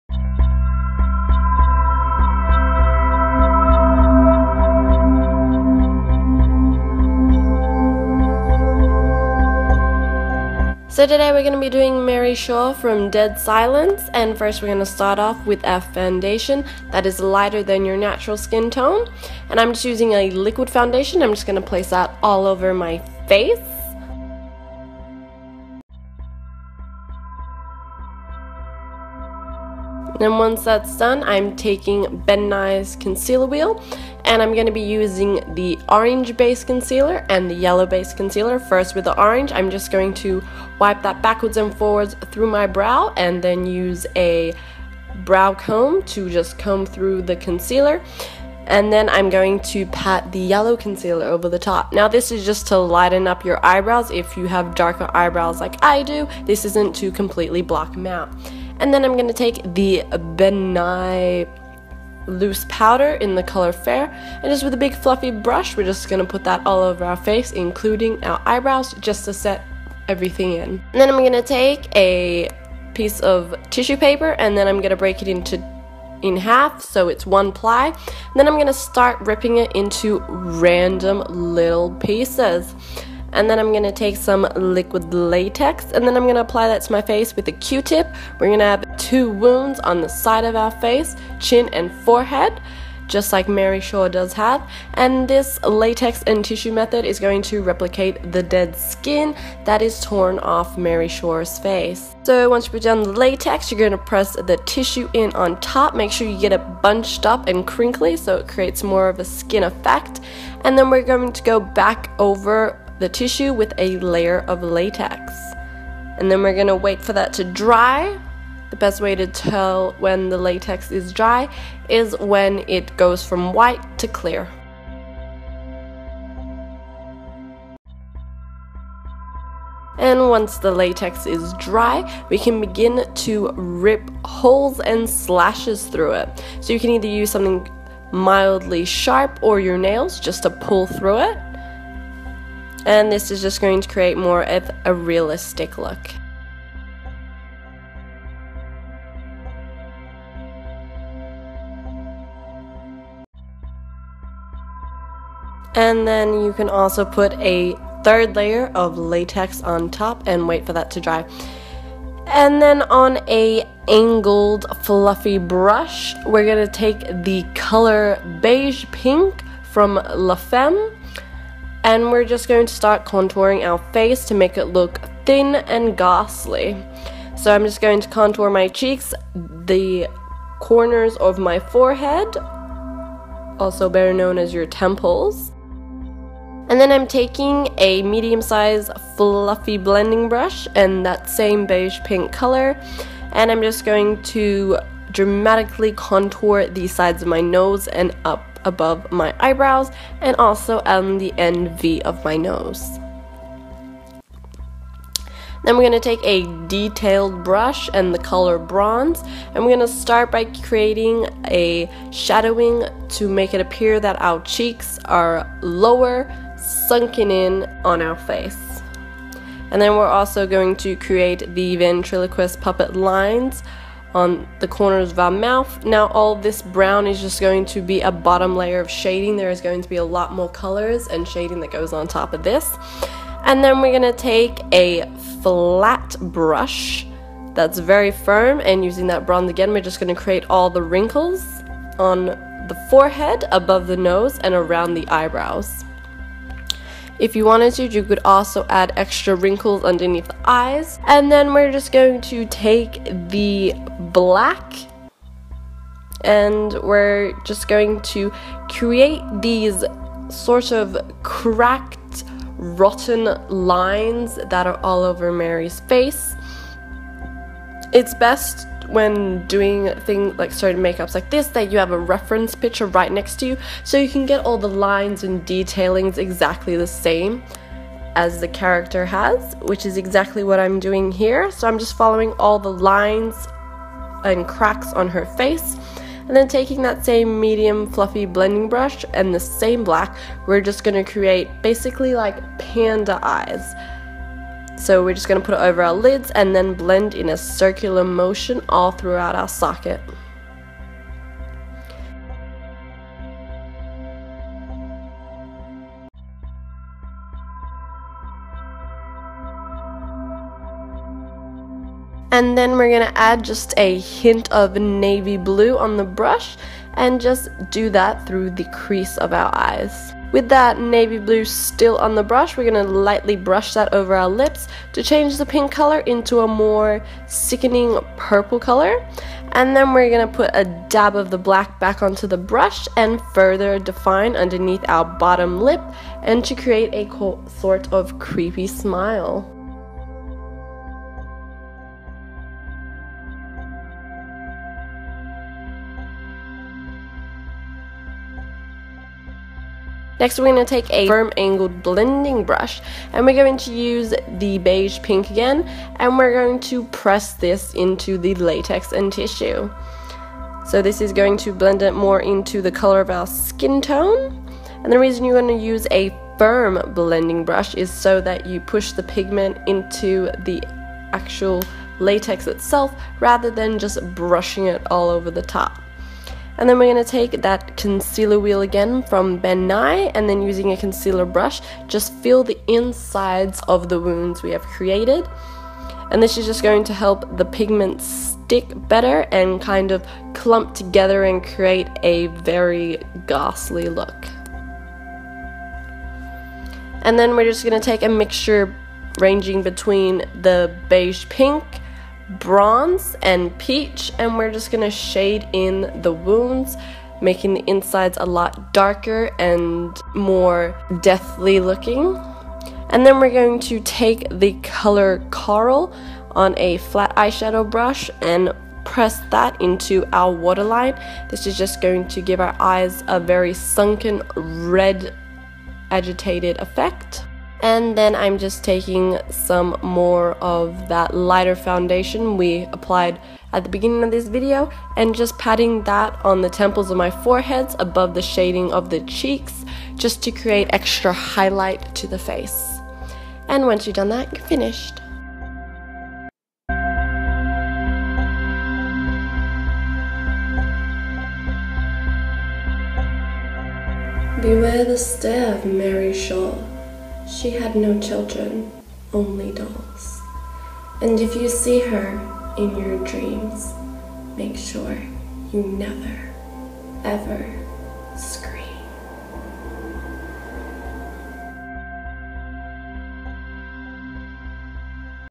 Mary Shaw - Dead Silence Makeup Tutorial
Character makeup inspired by Mary Shaw, from the film 'Dead Silence'.